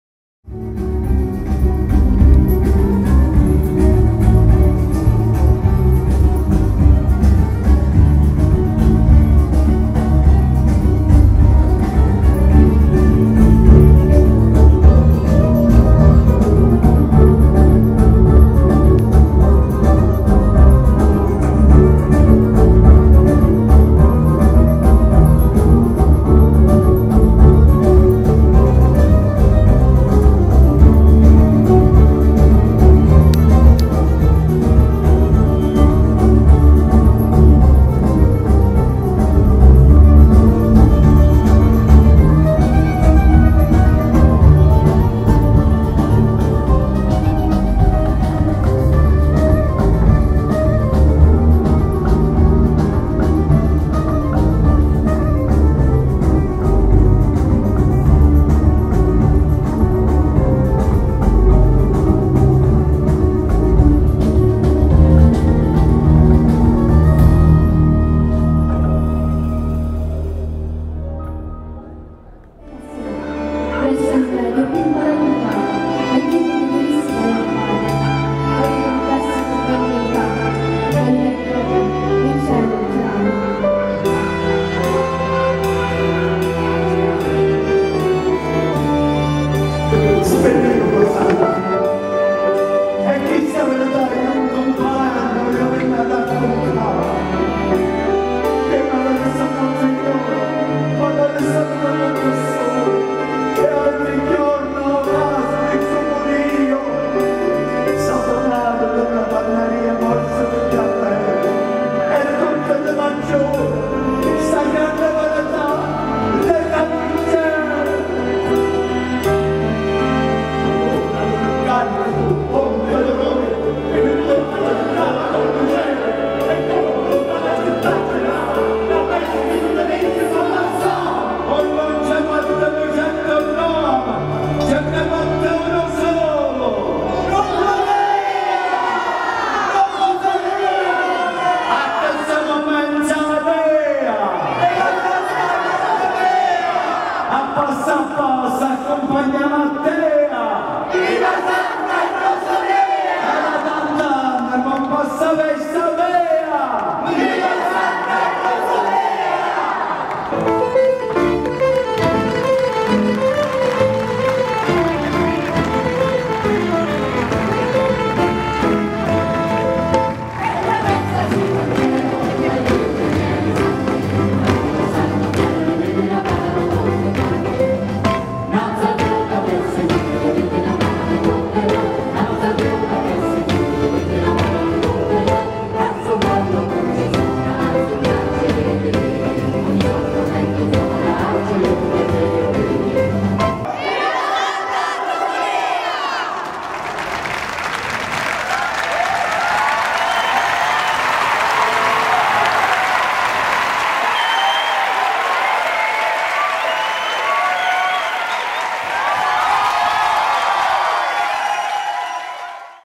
Palermo, 250mila in piazza per il 'Festino' della santa Patrona Rosalia
250.000 spettatori per il Festino dedicato alla Misericordia. Questa la stima degli organizzatori secondo i quali Palermo ha risposto con ardore alla sua festa più amata che stavolta è stata costruita in soli dodici giorni. Ma ha funzionato tutto: grazie ad un imponente servizio di forze dell’ordine, ma soprattutto con l’aiuto di centinaia di volontari.